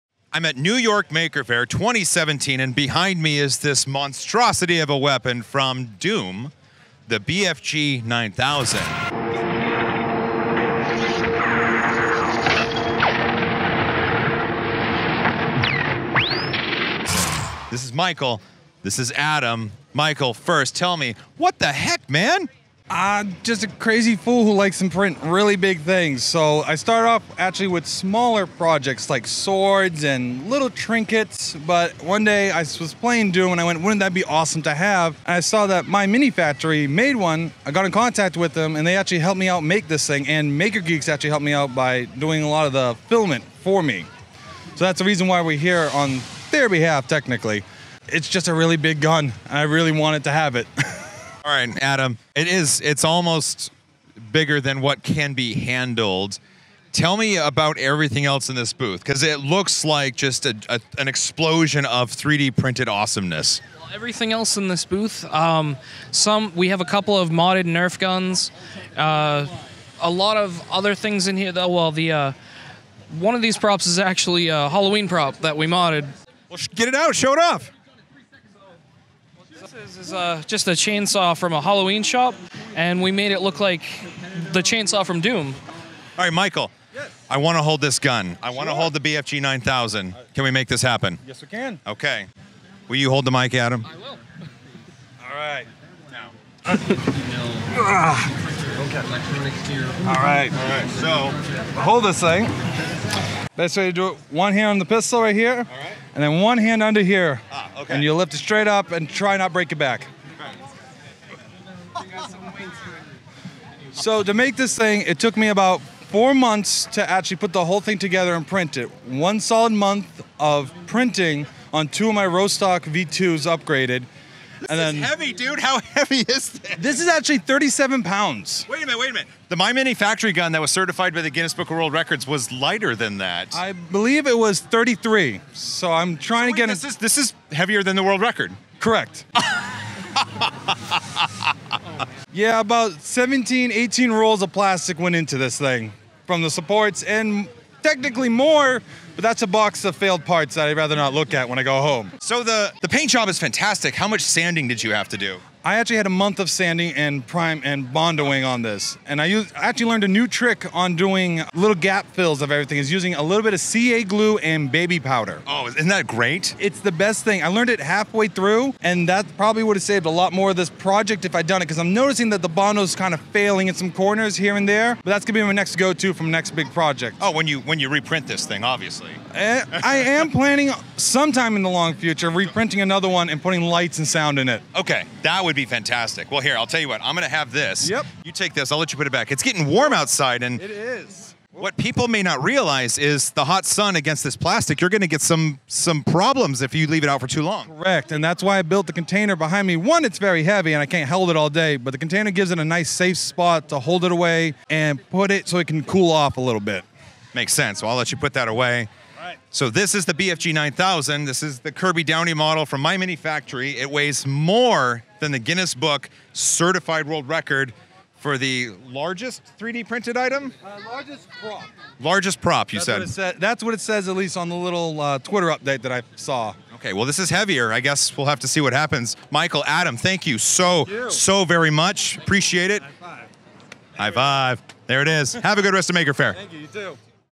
New World Record? 3D Printed BFG 9000 Gun from Doom / Maker Faire New York MFNY17 Midnight Giant
Michael and Adam of Midnight Giant DID NOT disappoint at Maker Faire New York 2017. A fully 3d printed BFG weighing in at 37 pounds, BEATING THE WORLD RECORD. It's insane and awesome.

== Cool New T-Shirts!

509 NE 165th st
Shoreline, WA 98155
USA